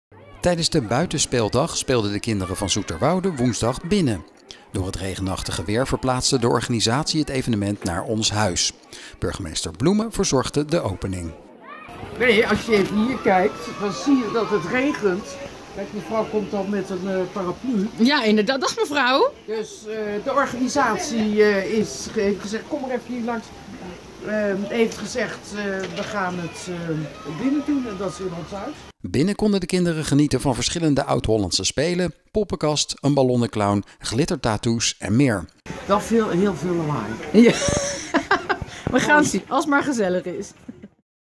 2019-06-12 Buitenspeeldag Zoeterwoude
Kort verslag van de buitenspeeldag in Zoeterwoude. Burgemeester Liesbeth Bloemen legt uit waarom er binnen wordt gespeeld.